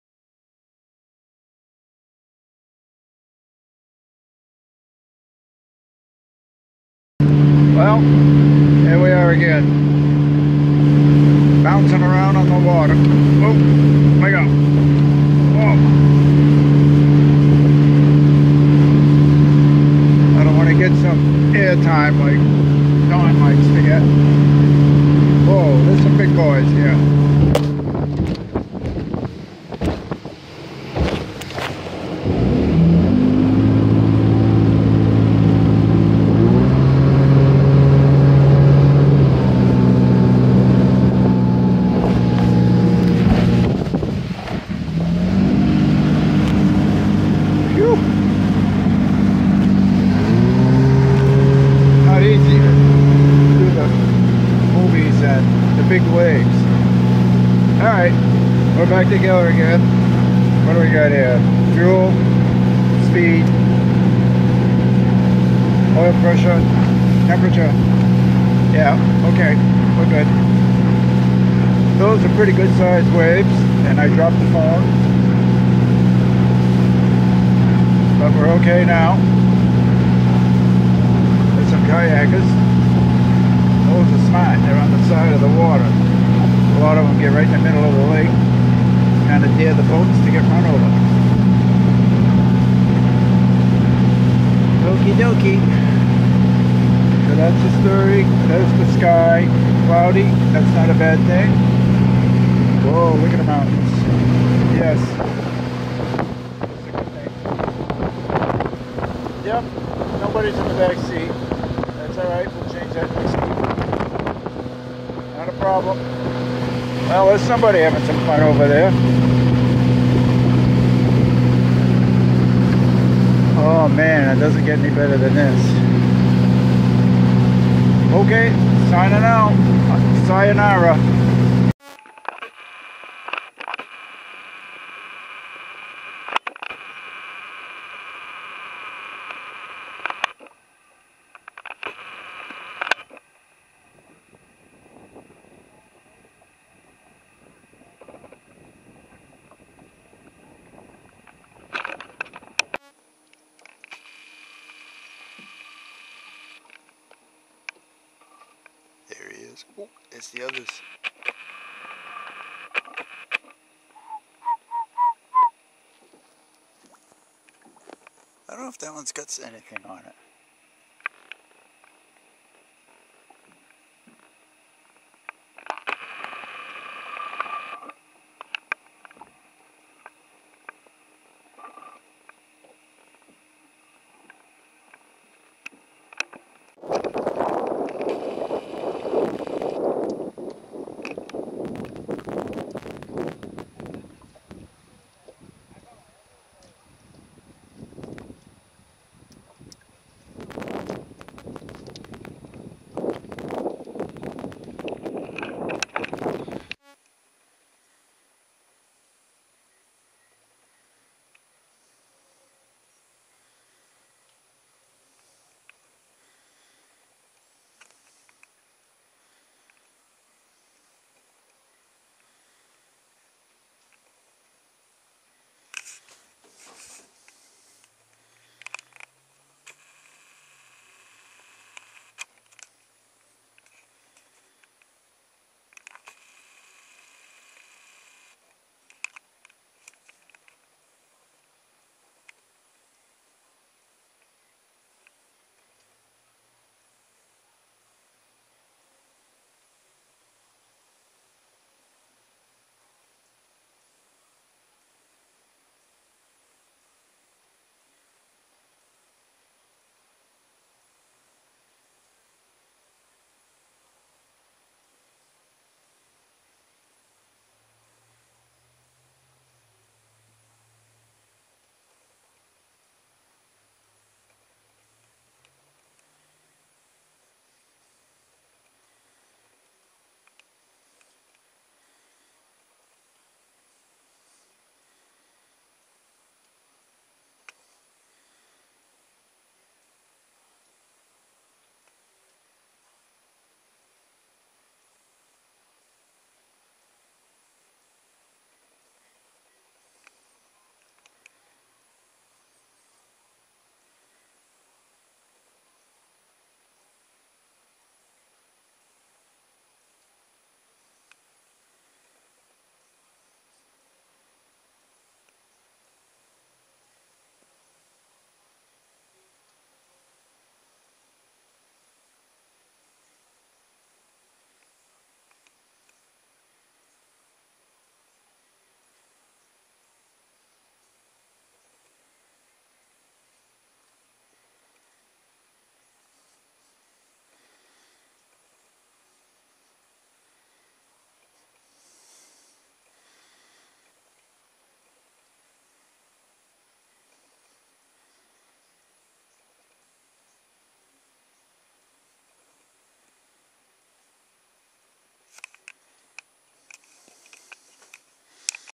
The boat, the loons, and the robin.
Take a ride in a Glastron SX-175 with a Volvo Penta 4.3 GL V6